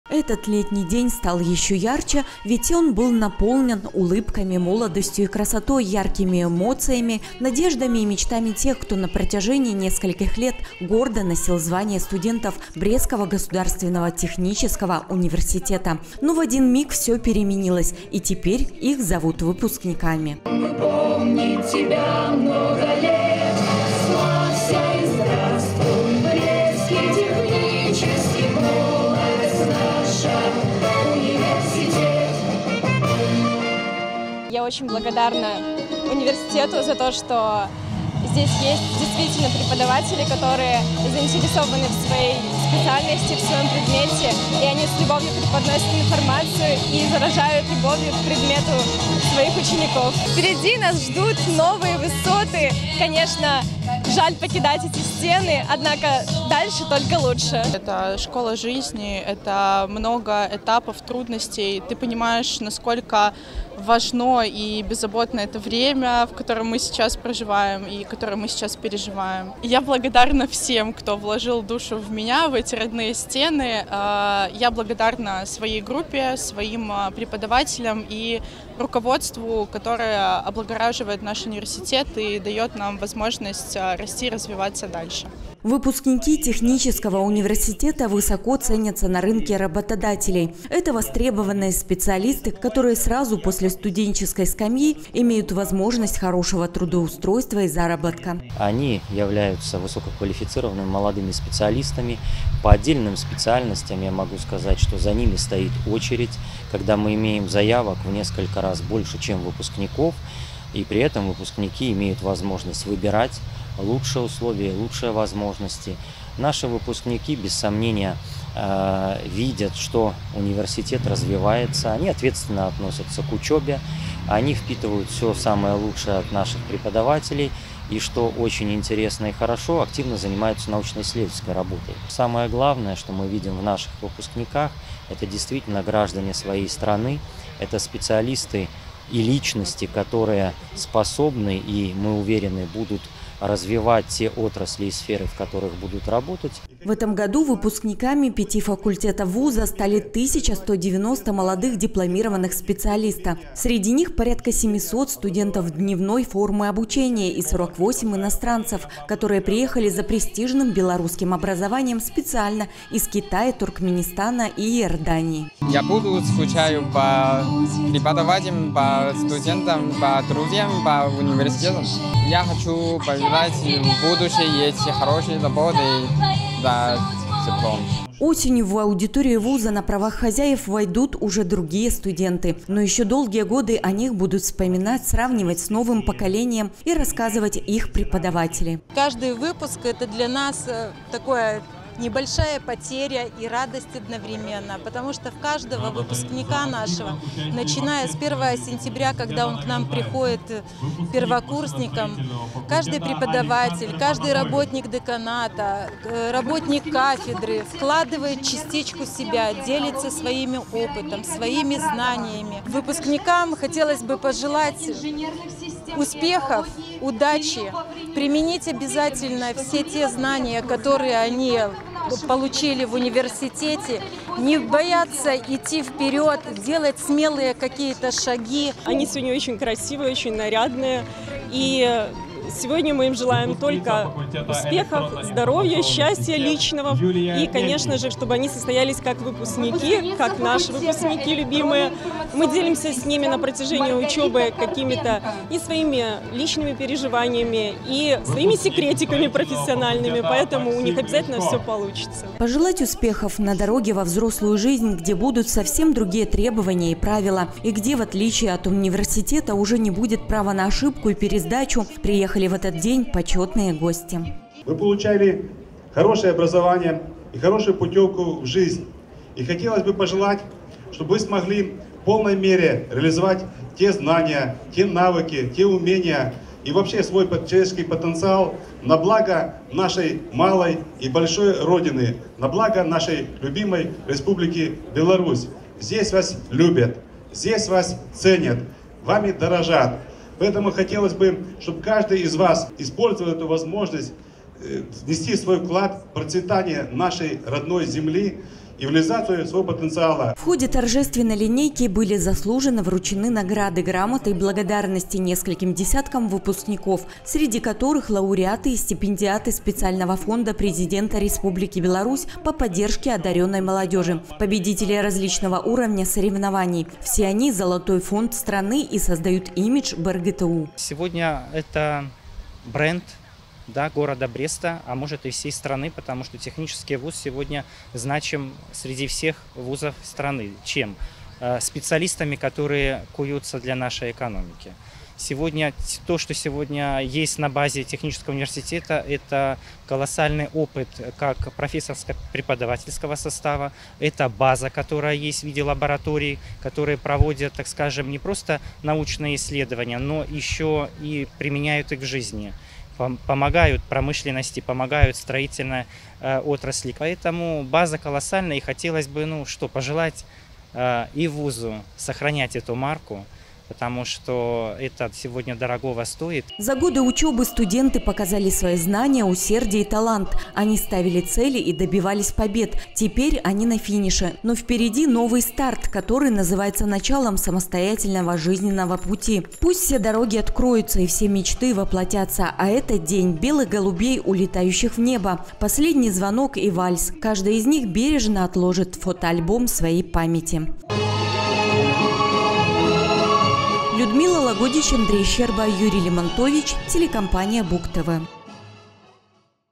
2023-06-09 Последний звонок» в БрГТУ.
В эти летние дни тысячи студентов покидают родные стены своих ВУЗов, чтобы словно лайнер, набравший за годы учёбы мощь и скорость, взмыть в небо новой, взрослой и самостоятельной жизни. Закрывается еще одна страничка летописи для сотен юношей и девушек под названием студенческая жизнь и в Брестском государственном техническом университете. Наша съёмочная группа побывала на самом ярком, трогательном и полном эмоций мероприятии - торжественной линейке, посвященной последнему звонку.